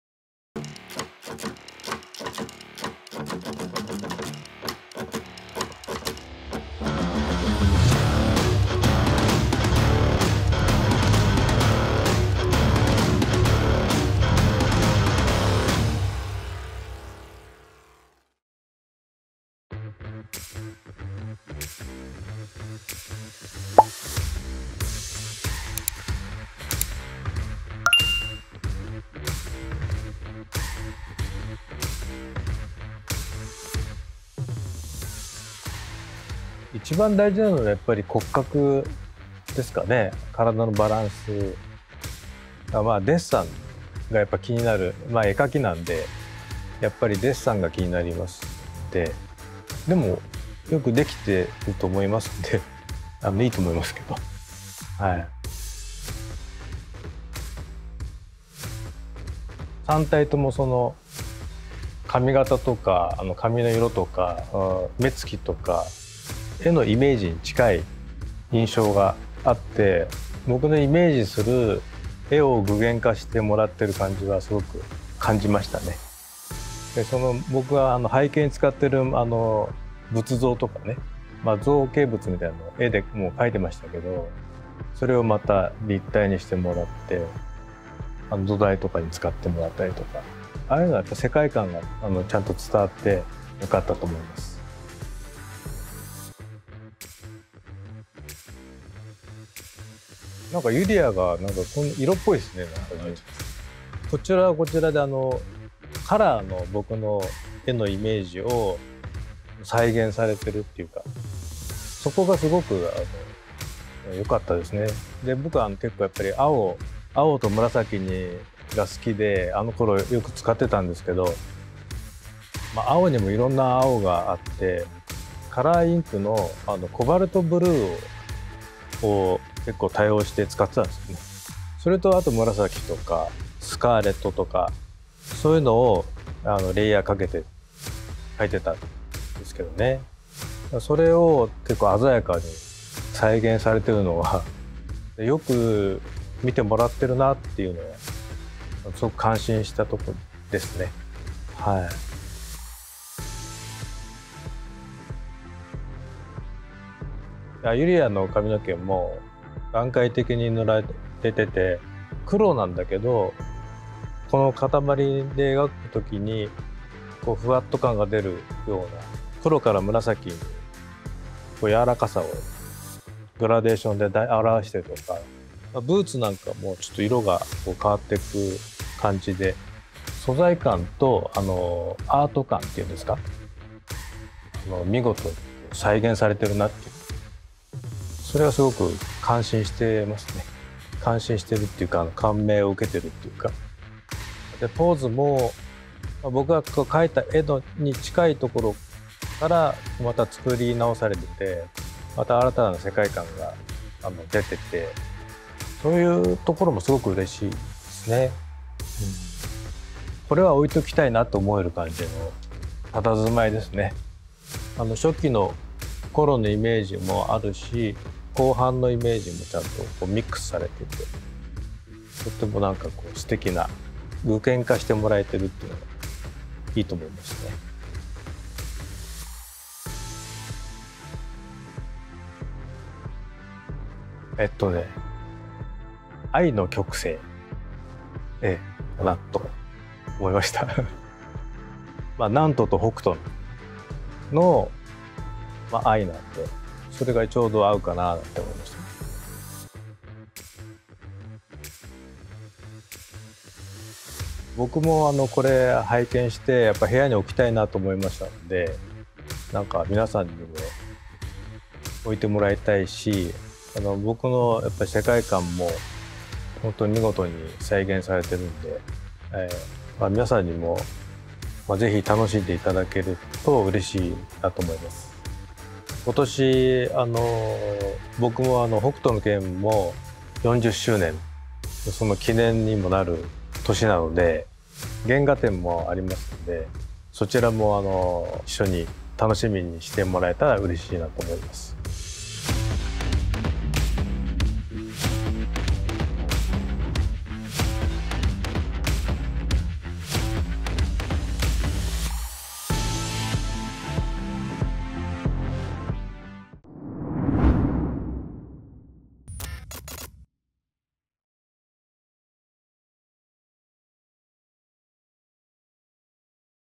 FIST OF THE NORTH STAR Tetsuo Hara Interview / UPMFOTNS-01S
Here is a special interview with the original artist of Fist of the North Star: Tetsuo Hara.
This interview was featured in our NEXT LEVEL SHOWCASE X PART THREE.

Thank you for joining us, Hara sensei!

Prime 1 Studio Next Level Showcase X

Enjoy and make sure to secure the order before they sell out!!